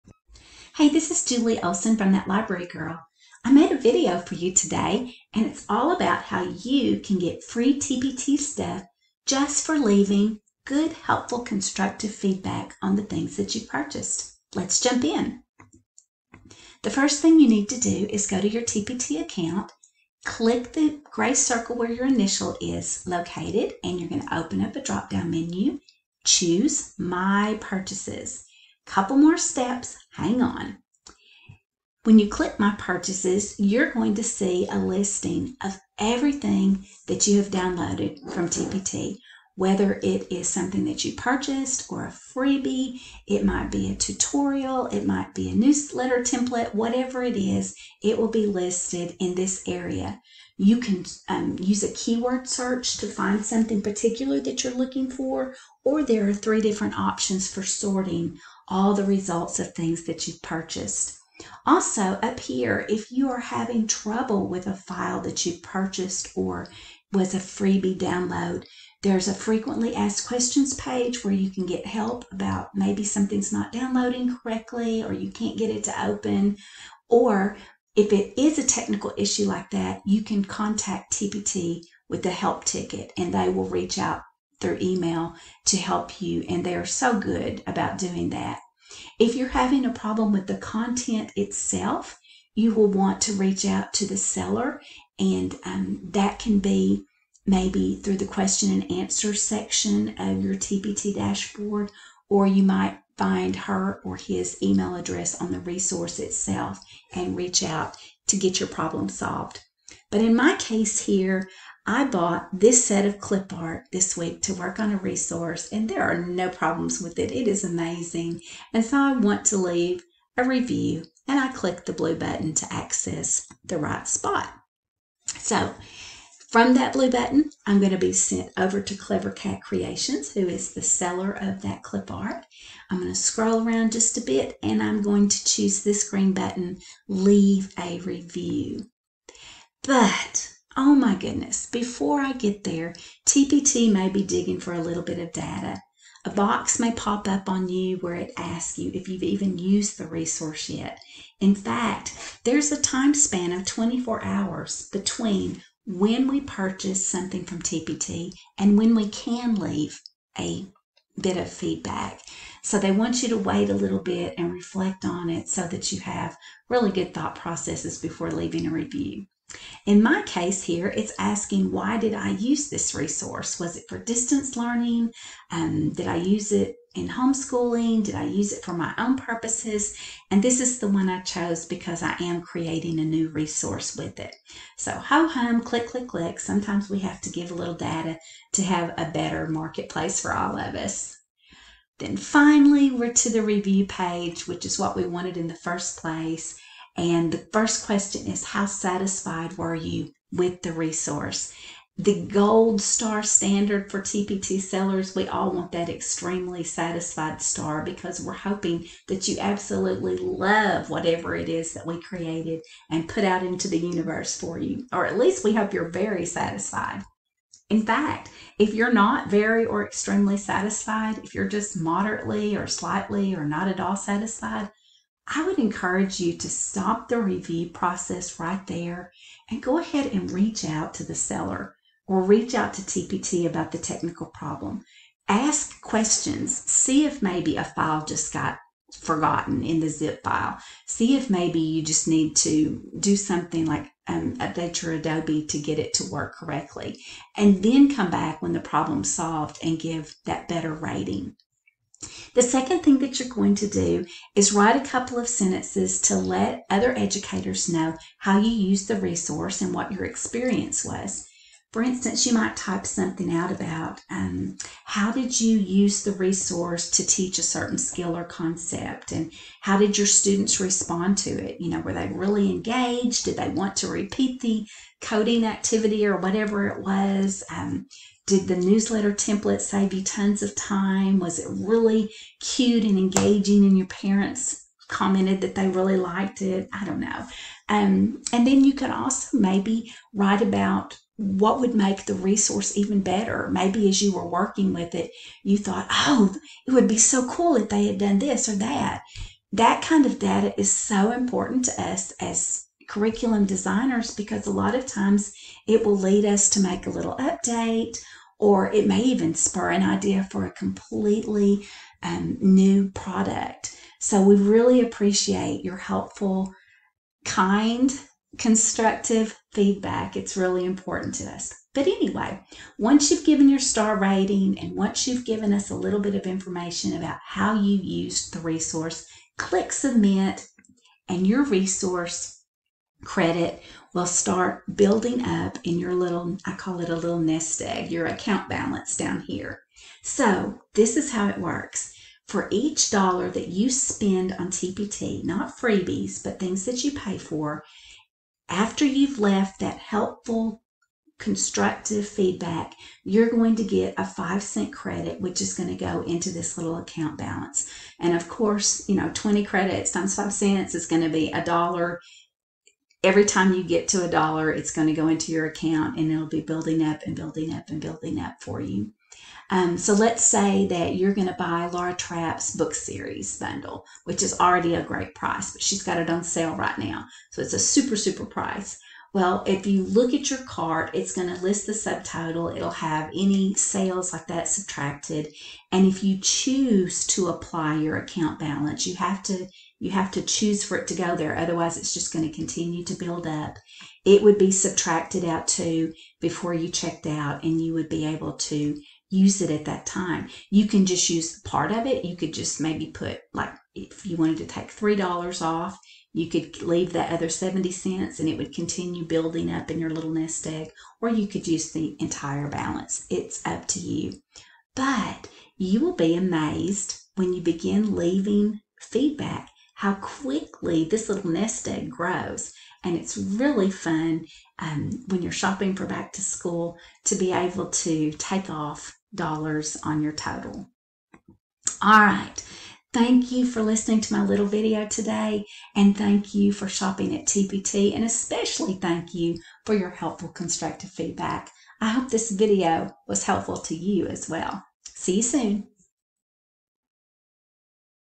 TpT Feedback and Credits
Explains the feedback process for gaining a little nest egg of Teachers Pay Teachers credits.